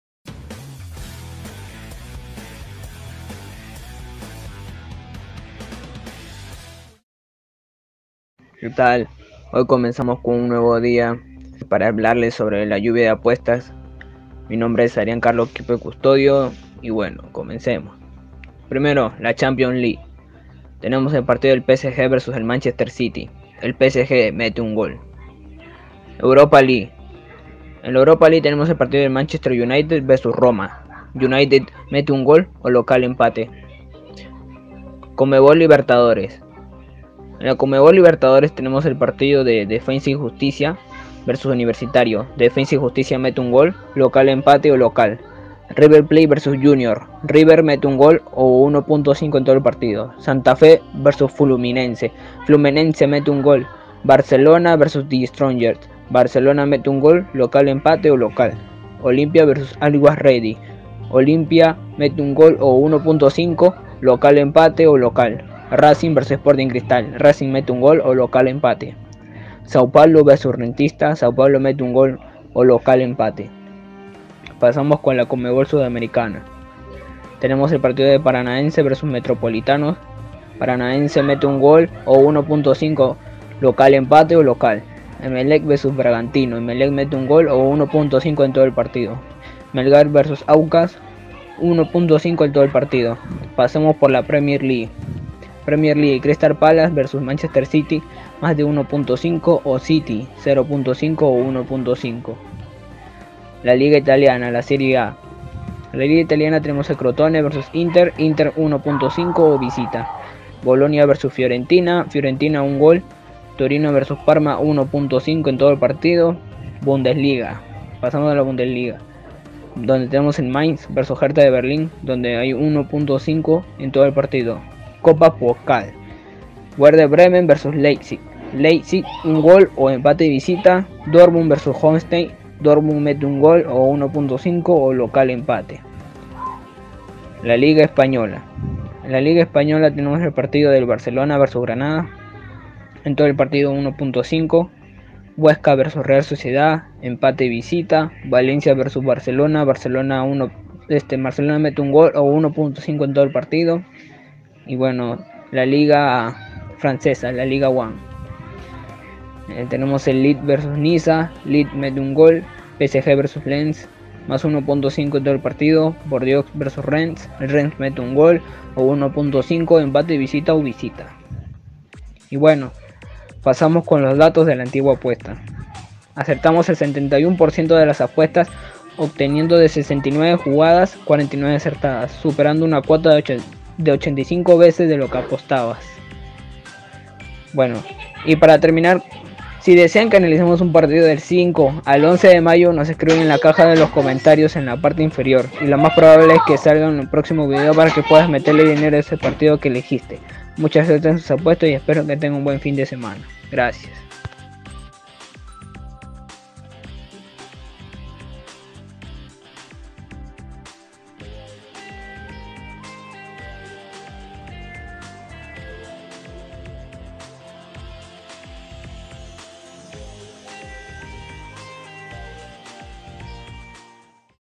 APUESTAS FIJAS para CHAMPIONS, LIGAS TOP, LIBERTADORES y MÁS || Lluvia de apuestas - 27/04/21
Apuestas deportivas de futbol para Champions League, Europa League, Premier League, LaLiga, Serie A, Bundesliga, Ligue 1, Copa Libertadores, Copa Sudamericana y más.